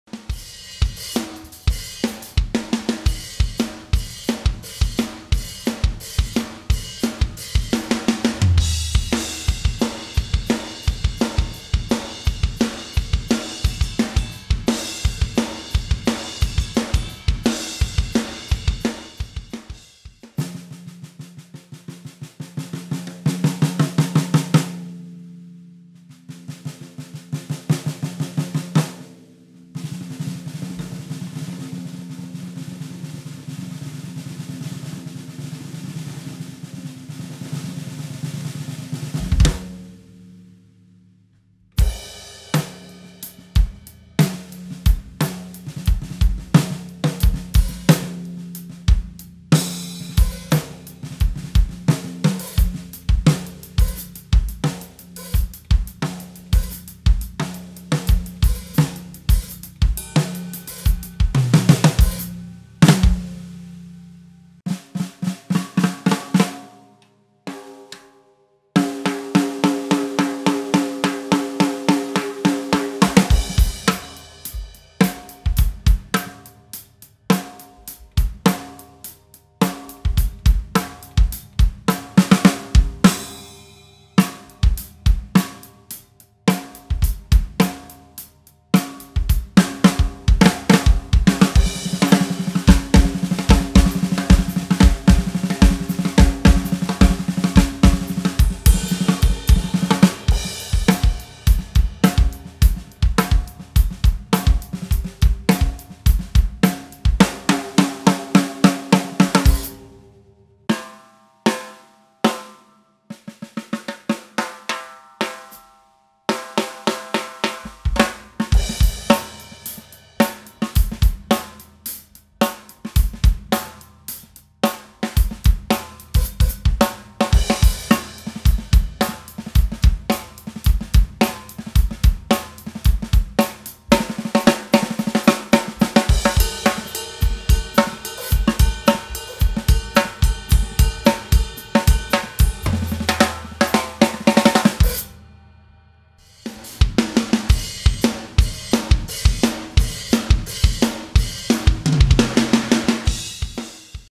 Stanton Moore Spirit of New Orleans Titanium Snare Drum 14x6.5 Galactic Black
Stanton Moore Spirit of New Orleans Titanium Snare Drum 14x6.5 Galactic Black - SM6514TI-GB

Drum set used in this video:
Gretsch USA Custom 4pc Satin Seafoam Green 22x14, 12x8, 14x14